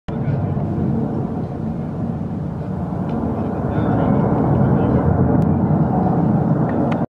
F/A-18C Hornet at Kaivari 21 (take #2)
F/A-18C Hornet (pilot Anssi Nieminen) at the Kaivopuisto Airshow 2021.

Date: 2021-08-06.

F/A-18C Hornet (ohjaaja Anssi Nieminen) Kaivopuiston lentonäytöksessä.